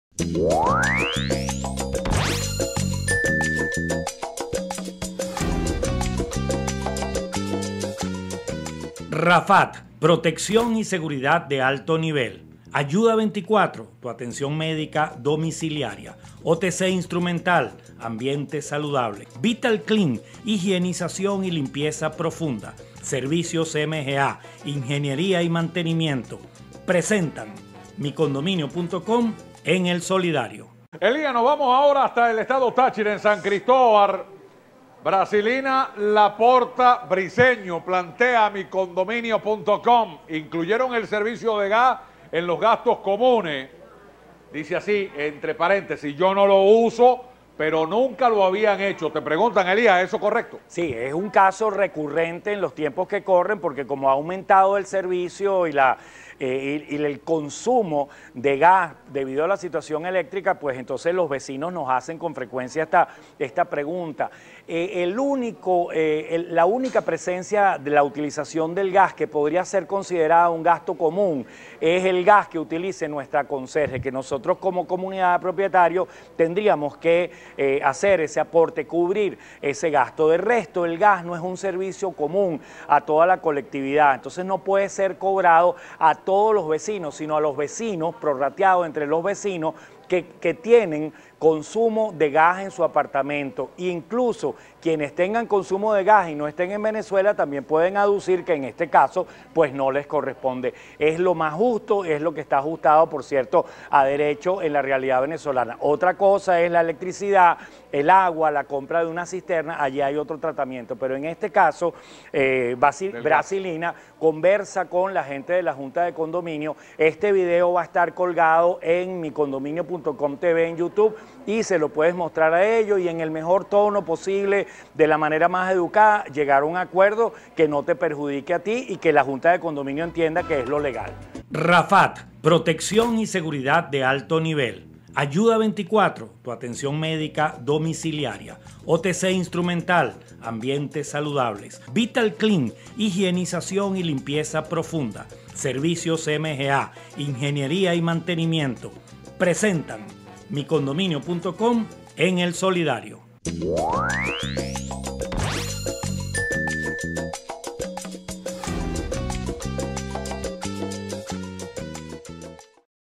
Nunca lo habian hecho y ahora incuye el servicio de gas, aunque no no lo uso,como gasto común.
Nunca lo habian hecho y ahora incuye el servicio de gas, aunque no no lo uso,como gasto común. ¿Es correcto?
 Caracas,Venezuela. Globovision. Miercoles 10 de Marzo 2021